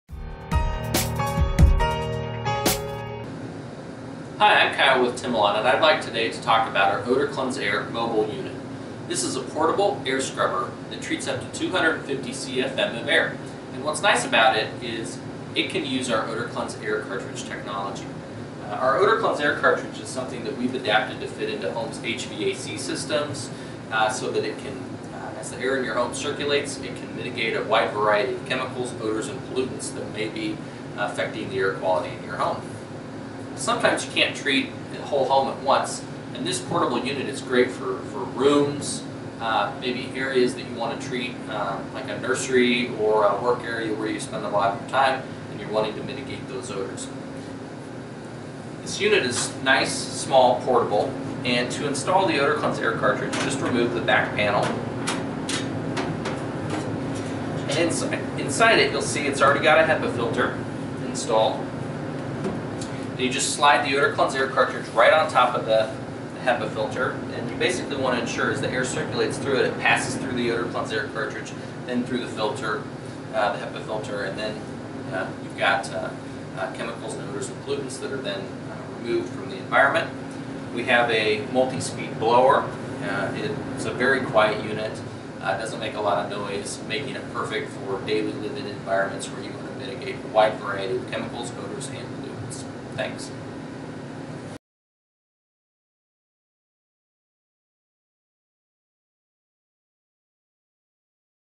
How to Reduce VOCs In your Home
The OdorKlenz-Air® Mobile is your solution to improving your homes indoor air quality by removing chemical odors, fragrances, VOCs, and allergens. The unit is portable and once the OdorKlenz HVAC filter is installed you will begin to remove odors in the air .

OdorKlenz-Air® Mobile utilizes hospital-grade technology to provide maximum air cleaning efficiency for your home and office. OdorKlenz-Air Mobile combines certified HEPA filtration along with our OdorKlenz-Air Cartridge to remove airborne pollen, dust, odors, mold, gases, fragrance, VOCs, allergens, bacteria, and more.

+ An easily installed OdorKlenz-Air Cartridge contains proprietary blend of materials to neutralize odors, chemicals, gases, and Volatile Organic Compounds (VOCs).

+ Individually certified true HEPA filter with 80 square feet of media removes harmful dust, particulate, allergens, pet dander, and more at 99.99 percent efficiency. Because there is no bypass around the HEPA filter, every unit performs at HEPA efficiency.
+ A four speed blower offers the quiet and efficient air flow. The low Whisp-Air speed allows for near silent operation while still maintaining odor elimination. Medium to High speeds create maximum air efficiency at noise levels suitable for residential and commercial use.